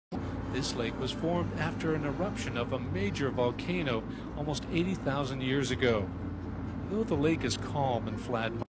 International Film & Video
A ridge of mountains towers over Lake Atitlan.